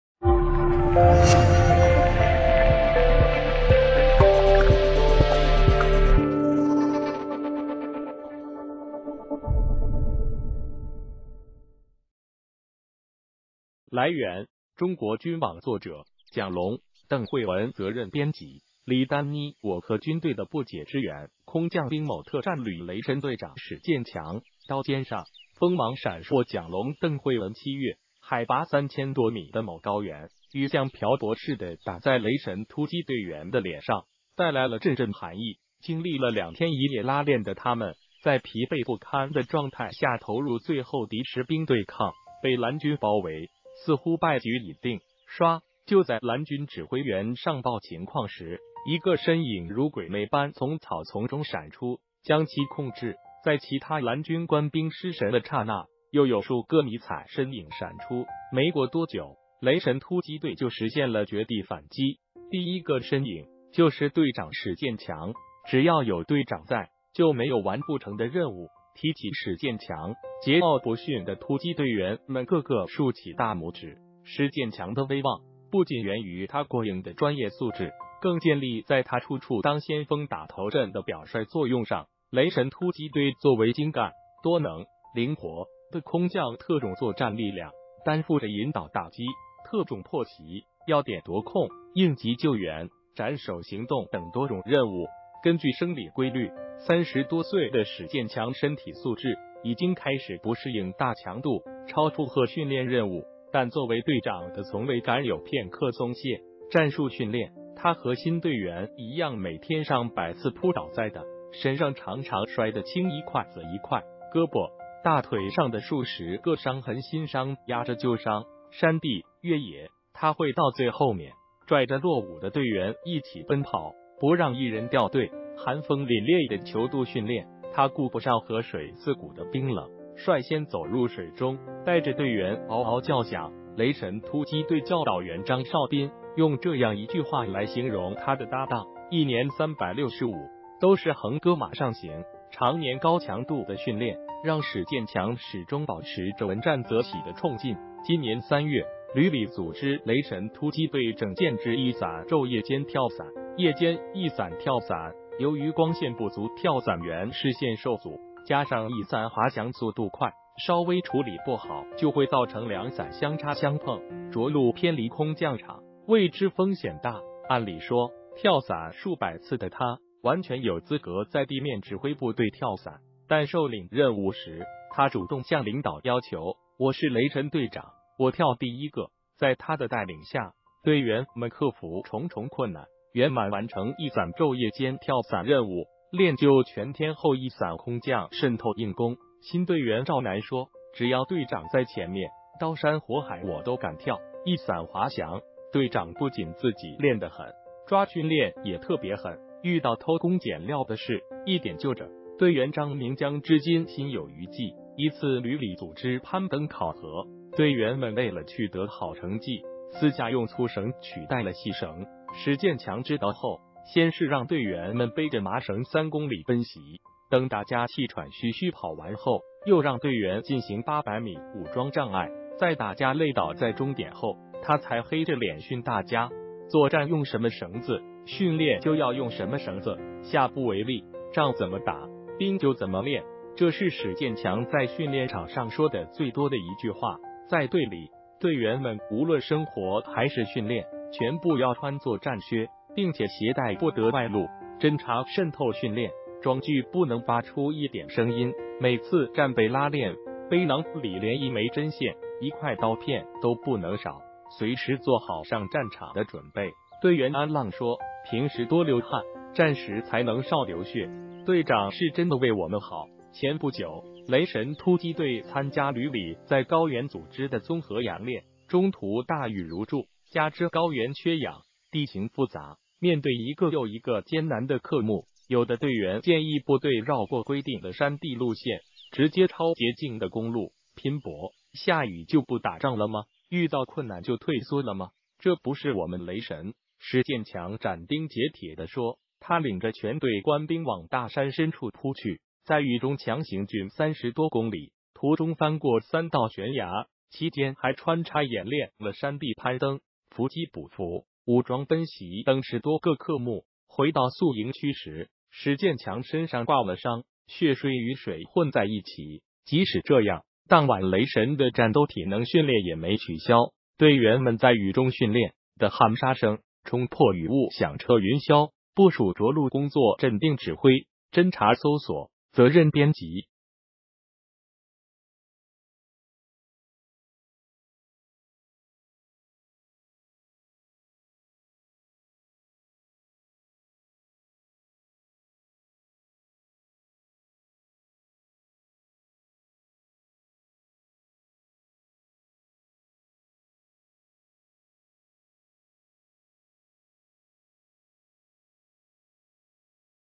只要有“雷神”队长在，就没有完不成的任务！
谢谢！来源：中国军网作者：蒋龙、邓惠文责任编辑：李丹妮我和军队的不解之缘空降兵某特战旅“雷神”队长史建强——刀尖上，锋芒闪烁蒋龙邓惠文7月，海拔3000多米的某高原，雨像瓢泊似的打在“雷神”突击队员的脸上，带来了阵阵寒意。经历了两天一夜拉练的他们，在疲惫不堪的状态下投入最后的实兵对抗——被“蓝军”包围，似乎败局已定。“唰！”就在“蓝军”指挥员上报情况时，一个身影如鬼魅般从草丛中闪出，将其控制；在其他“蓝军”官兵失神的刹那，又有数个迷彩身影闪出，没过多久，“雷神”突击队就实现了“绝地反击”。第一个身影，就是队长史建强。“只要有队长在，就没有完不成的任务。”提起史建强，桀骜不驯的突击队员们个个竖起大拇指。史建强的威望，不仅源于他过硬的专业素质，更建立在他处处当先锋打头阵的表率作用上。“雷神”突击队作为“精干、多能、灵活”的空降特种作战力量，担负着引导打击、特种破袭、要点夺控、应急救援、斩首行动等多种任务。根据生理规律，30多岁的史建强身体素质已经开始不适应大强度、超负荷训练任务，但作为队长的从未敢有片刻松懈。战术训练，他和新队员一样每天上百次扑倒在地，身上常常摔得青一块紫一块，胳膊、大腿上的数十个伤痕新伤压着旧伤；山地越野，他会到最后面拽着落伍的队员一起奔跑，不让一人掉队；寒风凛冽的泅渡训练，他顾不上河水刺骨的冰冷，率先走入水中，带着队员“嗷嗷叫响”……“雷神”突击队教导员张绍斌用这样一句话来形容他的搭档：一年三百六十五，都是横戈马上行。常年高强度的训练，让史建强始终保持着闻战则喜的冲劲。今年3月，旅里组织“雷神”突击队整建制翼伞昼夜间跳伞。夜间翼伞跳伞，由于光线不足跳伞员视线受阻，加上翼伞滑翔速度快，稍微处理不好就会造成两伞相插相碰、着陆偏离空降场，未知风险大。按理说，跳伞数百次的他，完全有资格在地面指挥部队跳伞，但受领任务时，他主动向领导要求：“我是‘雷神’队长，我跳第一个。”在他的带领下，队员们克服重重困难，圆满完成翼伞昼夜间跳伞任务，练就全天候翼伞空降渗透硬功。新队员赵楠说：“只要队长在前面，刀山火海我都敢跳。”翼伞滑翔。“队长不仅自己练得狠，抓训练也特别狠，遇到“偷工减料”的事，一点就着。”队员张铭江至今心有余悸，一次旅里组织攀登考核，队员们为了取得好成绩，私下用粗绳取代了细绳。史建强知道后，先是让队员们背着麻绳3公里奔袭，等大家气喘吁吁跑完后，又让队员进行800米武装障碍。在大家累倒在终点后，他才黑着脸训大家：“作战用什么绳子，训练就要用什么绳子，下不为例！”“仗怎么打，兵就怎么练”，这是史建强在训练场上说得最多的一句话。在队里，队员们无论生活还是训练，全部要穿作战靴，并且鞋带不得外露；侦察渗透训练，装具不能发出一点声音；每次战备拉练，背囊里连一枚针线、一块刀片都不能少，随时做好上战场的准备。队员安浪说：“平时多流汗，战时才能少流血，队长是真的为我们好。”前不久，“雷神”突击队参加旅里在高原组织的综合演练，中途大雨如注，加之高原缺氧、地形复杂，面对一个又一个艰难的课目，有的队员建议部队绕过规定的山地路线，直接抄捷径的公路。拼搏。“下雨就不打仗了吗？遇到困难就退缩了吗？这不是我们‘雷神’！”史建强斩钉截铁地说。他领着全队官兵往大山深处扑去，在雨中强行军30多公里，途中翻过三道悬崖，其间还穿插演练了山地攀登、伏击捕俘、武装奔袭等10多个课目。回到宿营区时，史建强身上挂了伤，血水雨水混在一起，即使这样，当晚“雷神”的战斗体能训练也没取消，队员们在雨中训练的喊杀声冲破雨雾响彻云霄。部署着陆工作镇定指挥。侦察搜索。责任编辑：